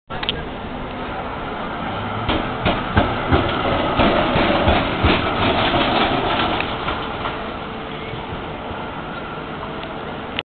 Z3 183 at Swanston/Bourke.
Z3 183 leaves Bourke Street tramstop heading to East Coburg on  route 1